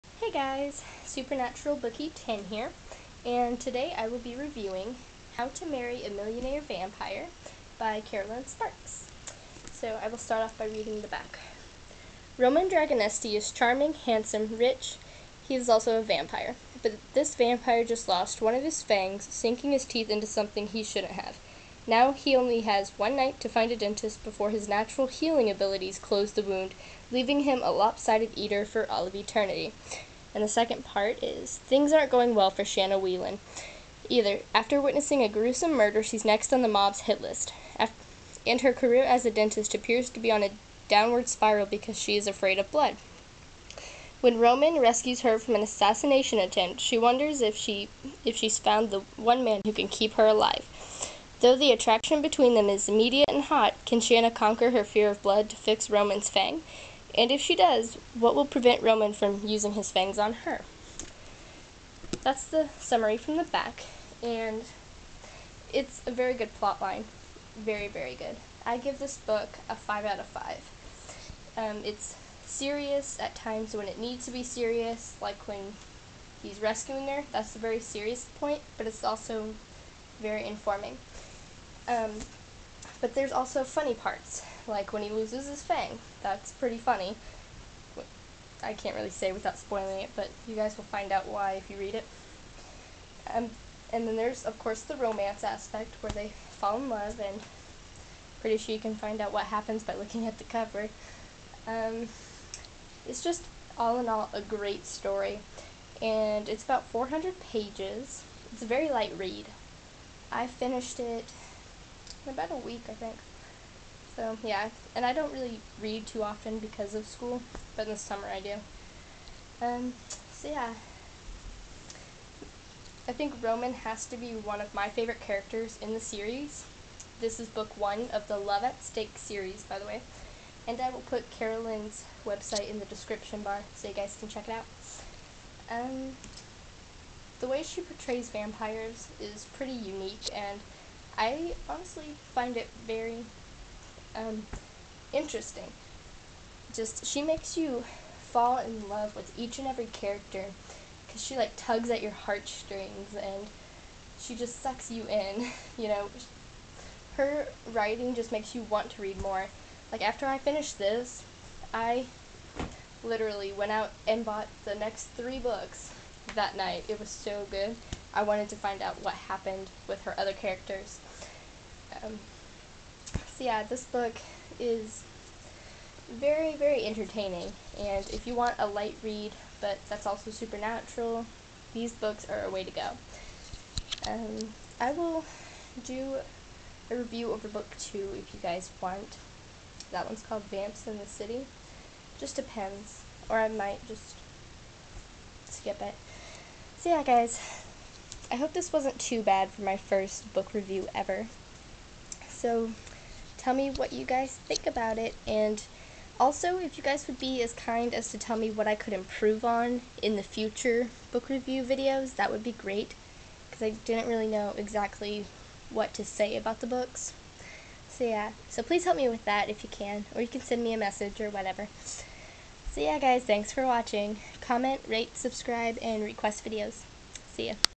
How to Marry a Millionare Vampire Book review!
This is my first book review. I hope that you guys enjoy it!